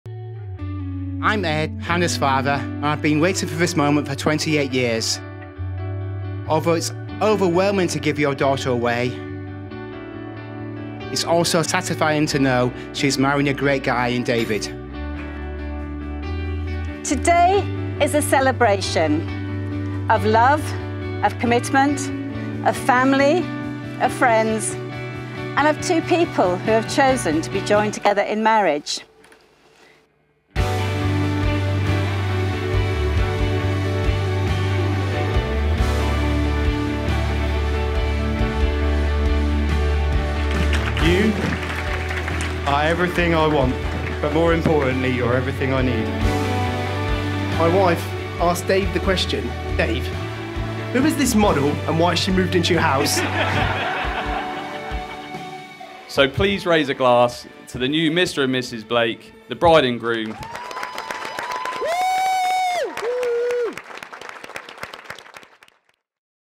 Hannah & Dave @ Pylewell Park - Wedding Film Trailer
What can I say about this Pylewell Park Wedding in Lymington, New Forest; IT WAS EPIC!
The bride looking like a model, the best groom speech I've ever heard, an outdoor ceremony from a movie set, a band that should be on the television and not in the background of an I Do Flix Wedding Film (which is still a cool achievement may I add), and quite possibly the best spaghetti bolognese I've ever tasted. And lets not forget a cutest little baby Rory looking so amazing!
We also had such an incredible team of suppliers as well all working together to make the day as amazing (and I truly mean amazing) as it was. Shout out to my second shooter, Chris Spice from Spice Wedding Films, Az from LemonTree Photography and his second shooter Drew Buckler Weddings.